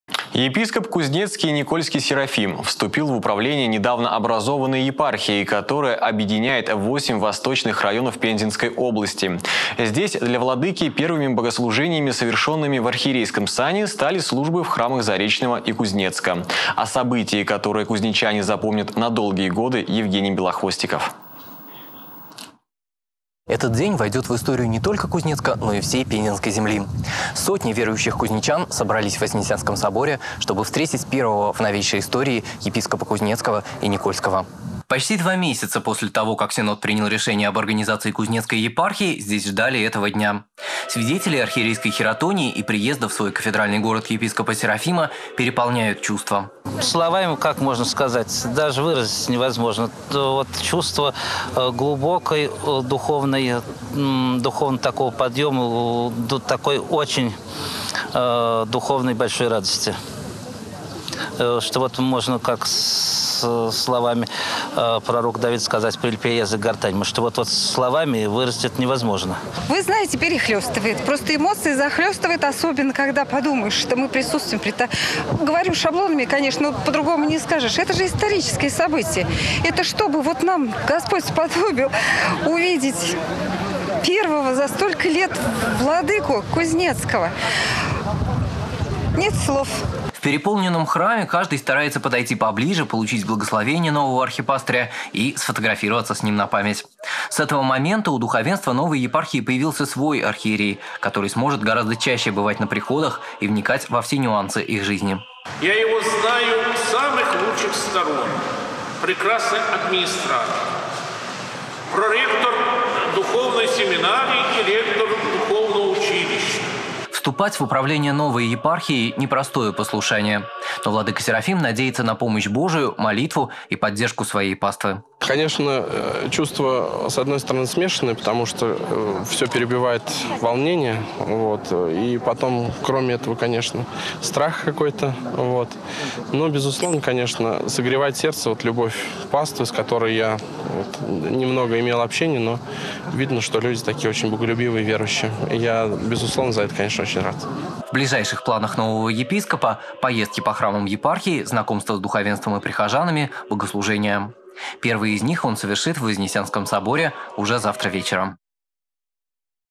Служение епископа Кузнецкого и Никольского Серафима
Епископ Кузнецкий и Никольский Серафим вступил в управление недавно образованной епархией, которая объединяет восемь восточных районов Пензенской области. Здесь для владыки первыми богослужениями, совершенным в архиерейском сане, стали службы в храмах Заречного и Кузнецка.